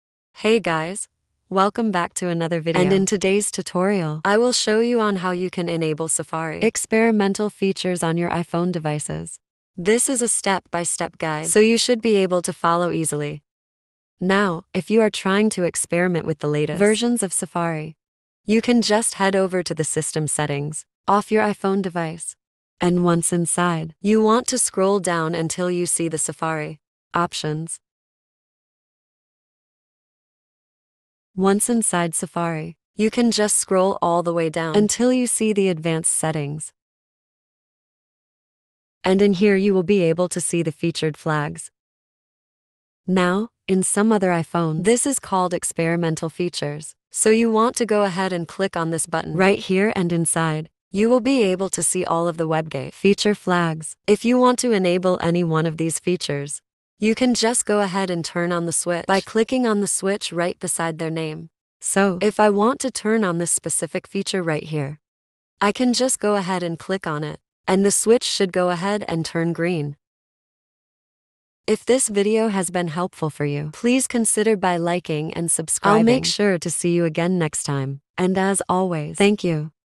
How To Enable Safari Experimental Features On iPhone
Are you curious about the cool experimental features that Safari has to offer on your iPhone? Then this video is perfect for you! Learn step-by-step instructions on how to enable these cutting-edge features and make your browsing experience even more enjoyable. With just a few taps, you can unlock all the extra functions. Don't miss out; watch our video now and stay ahead of the game with Safari's newest innovations.